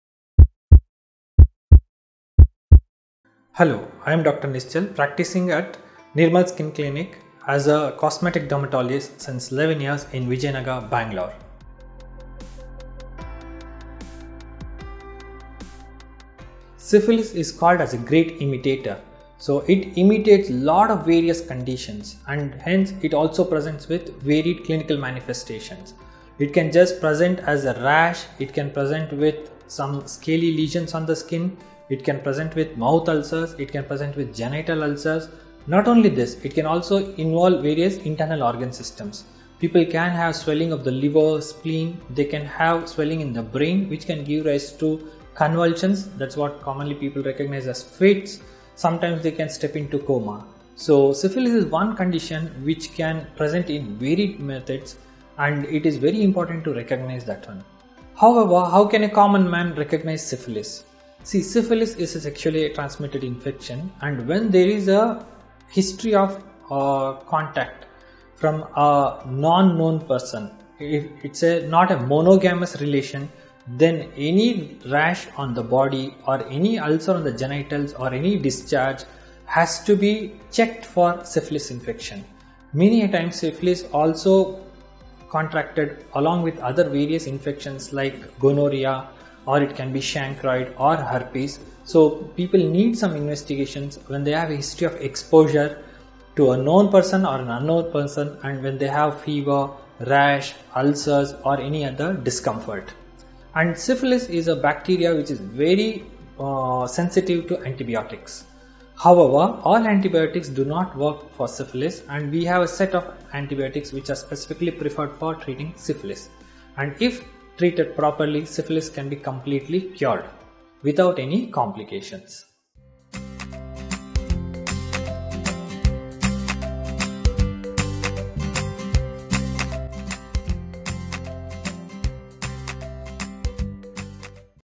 Signs of Syphilis in men & its management - Dr. Nischal K
Syphilis is called as a great imitator. So it imitates various clinical conditions and it presents  with several manifestations. It can present with a rash, with scaly skin, mouth ulcers, can present with genital ulcers. Not only this, it can involves swelling of various internal organs. People can have swelling of the liver, spleen. They can have swelling in the brain which can give rise to convulsions, which is commonly recognized as fits. Sometimes they can step into coma.  Syphilis is one condition which can present in many methods and it is very important to recognize that. How can a common man recognize  Syphilis? Syphilis is  a sexually transmitted infection and when there is a history of a relation from an unknown person, it is  not a monogamous  reaction, then any ulcer on the  body, or  any infection has to be  checked for  Syphilis  many at times, Syphilis is  also contracted at the time of  other various infections like gonorrhea, herpes, cancroid. So people require other various investigations when they have a history of contact with an unknown person and they have history of fever, rash, ulcers or any other discomfort. Syphilis is a bacteria, which is very sensitive to antibiotics. But we have  a  set of  antibiotics which are  specifically for  treating  Syphilis  and if treated properly,  Syphilis can be completely cured without  any complications.